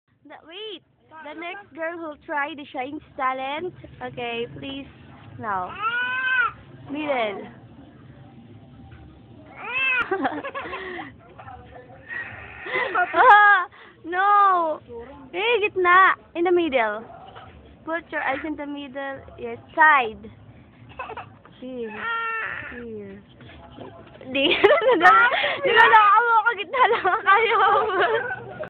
Special talent MOSICHICA MHENG VERSION
THIS GIRL named MHENG is trying to do SHINE A.K.A MOSICHICA special talent the crossing of the eye.. well she's not that good HAHAHAHA!